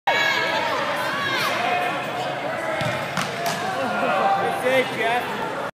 Great leaping save by the Goodfellas gk
Jeff with a tremendous diving save and quick reaction time saves this shot by the Sweatpeas